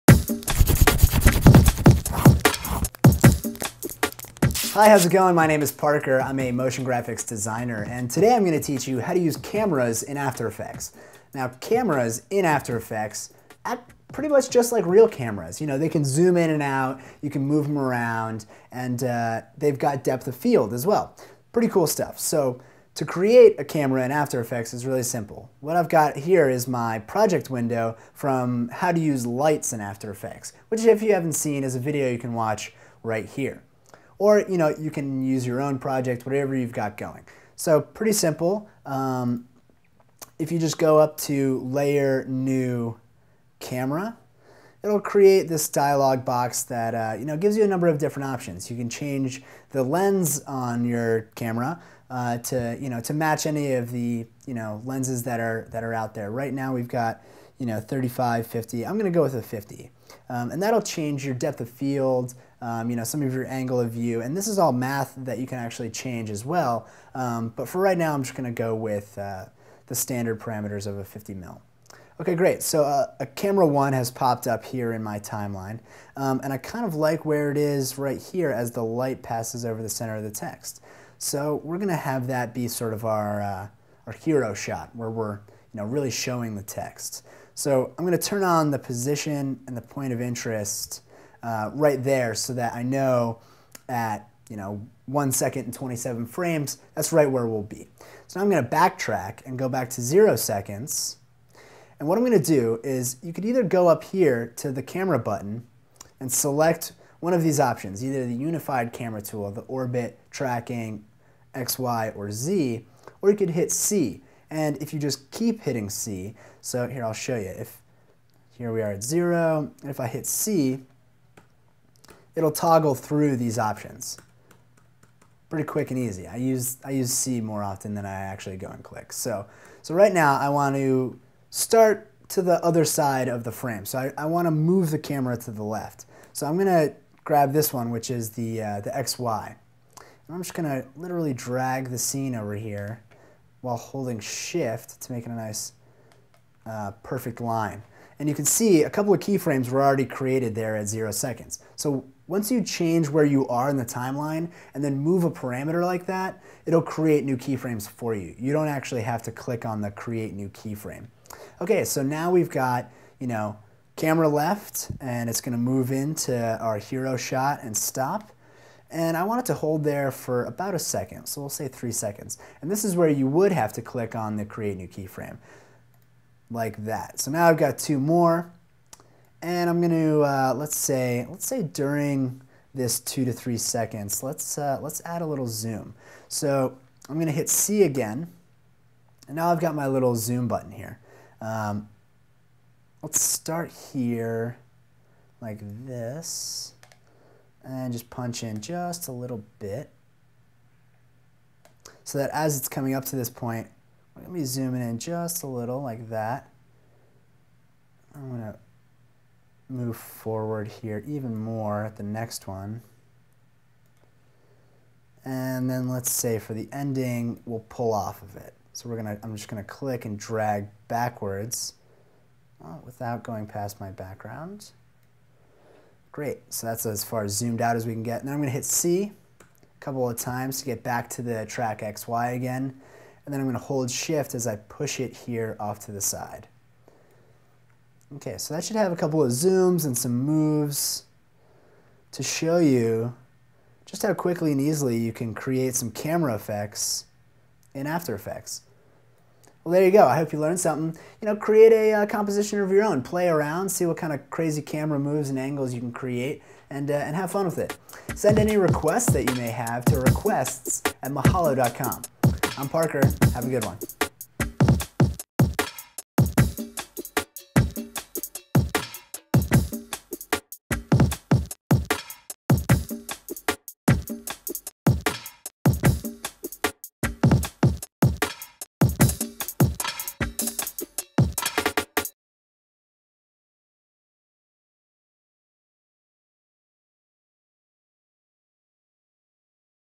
How to Use Cameras In Adobe After Effects
Cameras within Adobe After Effects work very similarly to cameras in real life. You can move them around and zoom in and out with them, as well as focus on specific areas with depth of field settings.

To create a Camera in After Effects, navigate to the top menu bar and click Layer -- New -- Camera. This will reveal the Camera Settings dialog box in which you can customize a few different options.

You can change the lens focal length Preset to 15mm, 20mm, 24mm, 28mm, 35mm, 50mm, 80mm, 135mm, 200mm or your own Custom setting. You can also Enable Depth of Field, Lock to Zoom and choose how you want to Measure Film Size.

Once your Camera is created, feel free to work with it. Click and hold on the Camera Tool button in the top tool bar and choose the Track XY Camera Tool.

With this camera chosen, press and hold Shift while clicking and dragging your clip to the left in the Composition window.

Scrub the play marker forward in the Timeline and click on the Create New Keyframe icon located on the far left side of the bottom panel to the left of the Timeline. With this new keyframe set to occur later in your Timeline, select the Track XY Camera Tool again and click and drag your clip to the right in the Composition window. This will cause the camera that's applied to your clip to move left to right, similar to panning or tracking sideways.

Feel free to experiment and play around with the various camera options and settings to create your own effects and movements.